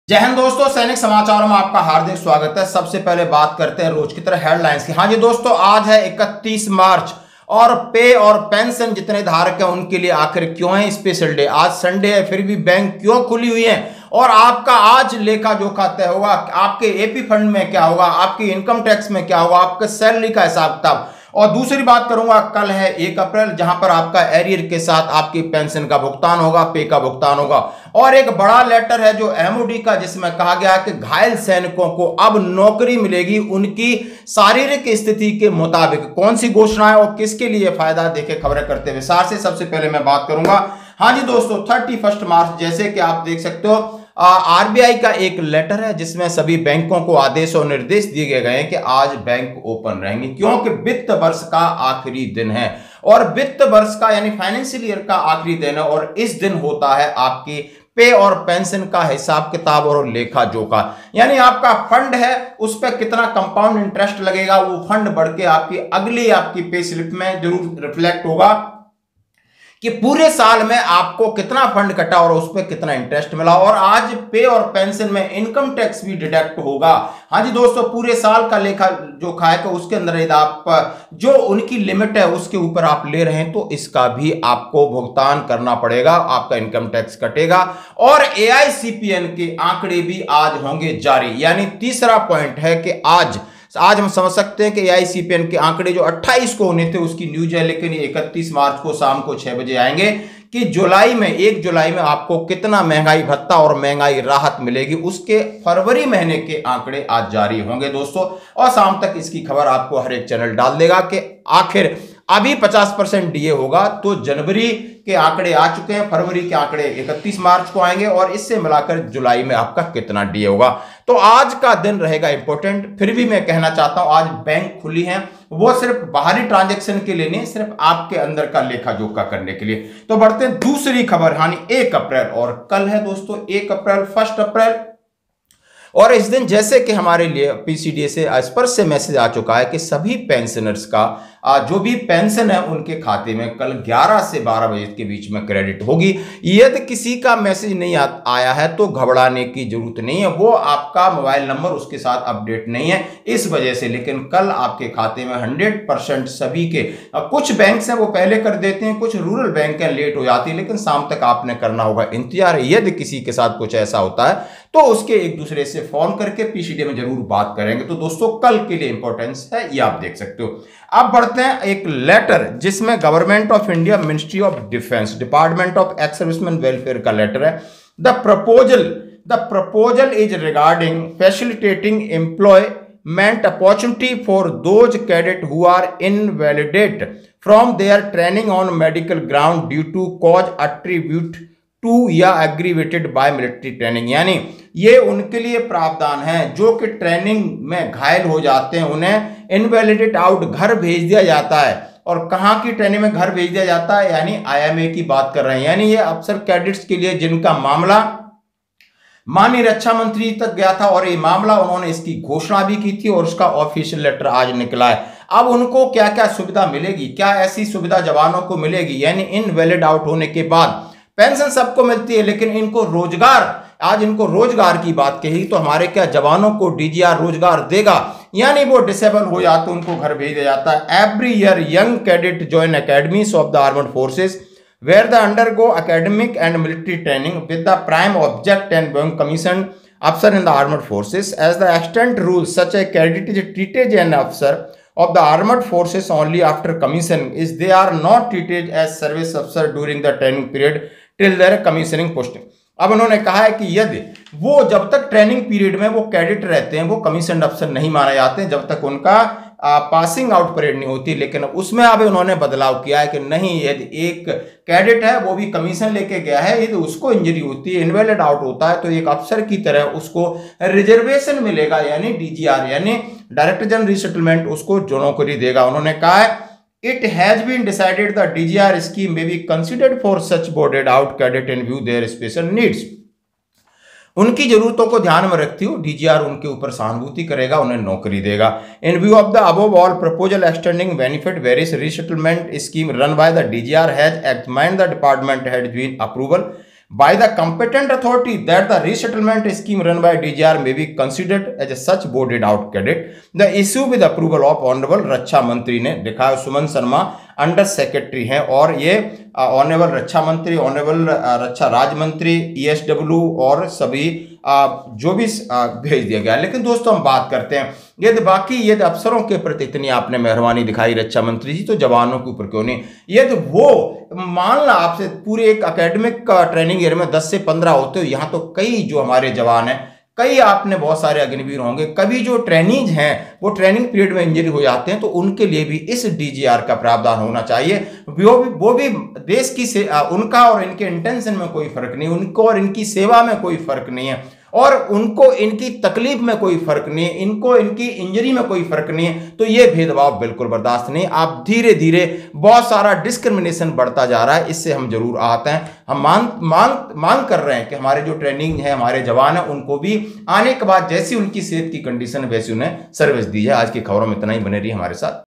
MoD की Policy सैनिकों को नौकरी, AFPP Fund,DA के आंकड़े, Income tax, कल Arrear/पेंशन का भुगतान
MoD की नई Policy सैनिकों को नौकरी; आज आखिरी दिन, AFPP Fund पर ब्याज,DA के आंकड़े, Income tax पर बड़ी Update और कल Arrear के साथ पेंशन का भुगतानMoD की New Policy घायल सैनिकों को मिलेगी नौकरी,पेंशन/Arrear के साथ 12:00 बजे 1 अप्रैलआज केंद्रीय कर्मचारियों के लिए DA के आंकड़े, AFPP फंड पर ब्याज और इनकम टैक्स का आखिरी दिन,Follow the Sainik Jeevan channel on WhatsApp